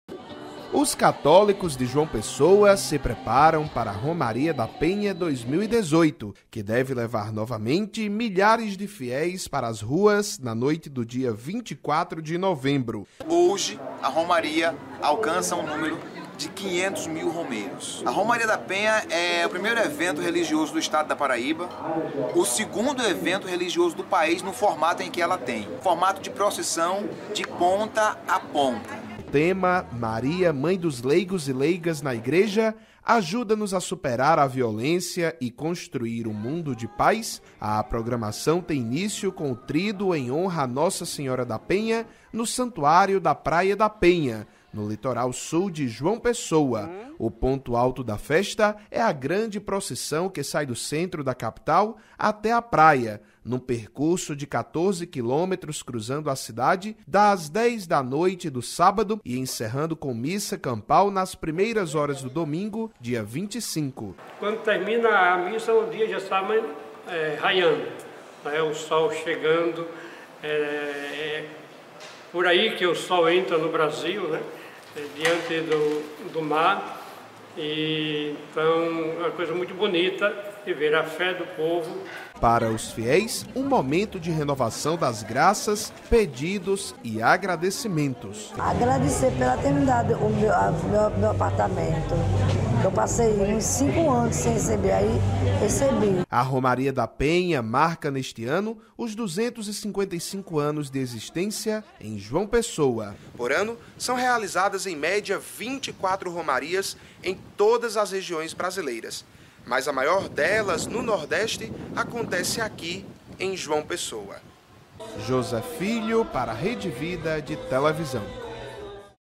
CONFIRA OS PREPARATIVOS PARA A SEGUNDA MAIOR PROCISSÃO CATÓLICA DO BRASIL - JCTV - 12/11/2018
OS  PREPARATIVOS PARA A ROMARIA DE NOSSA SENHORA DA PENHA, JÁ COMEÇARAM NA PARAÍBA . A ROMARIA É CONSIDERADA A SEGUNDA MAIOR PROCISSÃO CATÓLICA DO BRASIL.